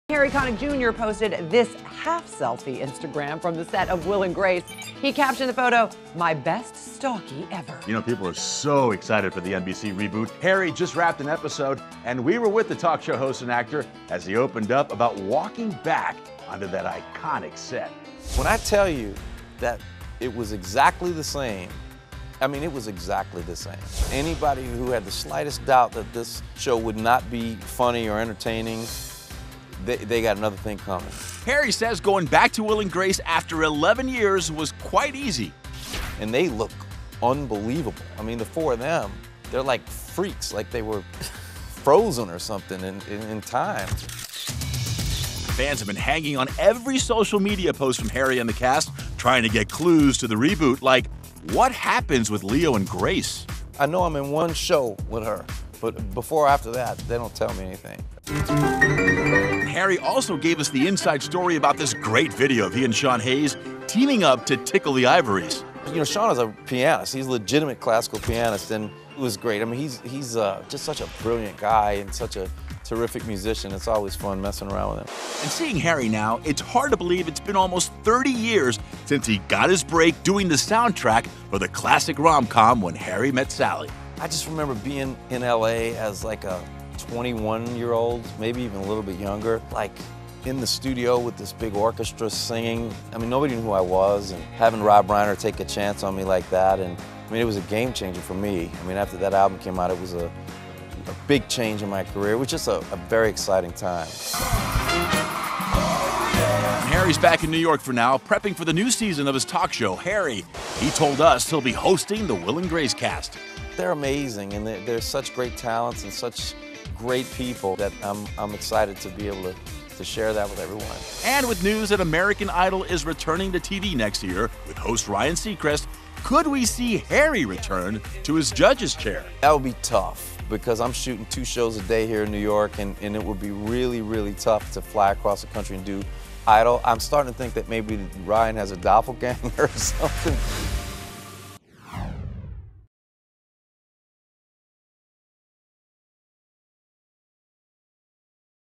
Harry Connick Jr. On Returning to 'Will and Grace' After 11 Years: 'It Was Exactly the Same'
The Insider caught up with the 'Harry' host to chat about his big return to the NBC reboot.